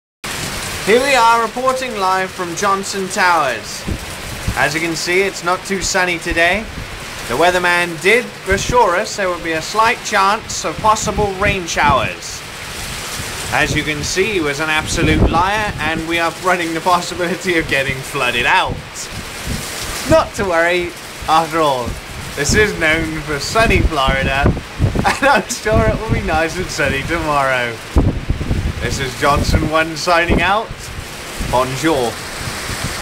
Hurricane in Florida
Bonjour all,

Im sorry about this video guys, I couldnt resist! It was an opportunity I couldnt pass up on. I was making light of a somewhat rough time. I am sure you all heard of and in some cases went through the events of Tropical Storm Fay, in September.

 Ohh the virtues of living in Florida. And people say they come on holiday to Florida to escape the English bad weather.. Umm, clearly the tourist information is telling porkpies ;)

Hope you enjoy the video. Tell me if you like it.

Okay well I hope you find this video helpful, remember to shoot me any emails with questions and look out for my next video.

Englishtwist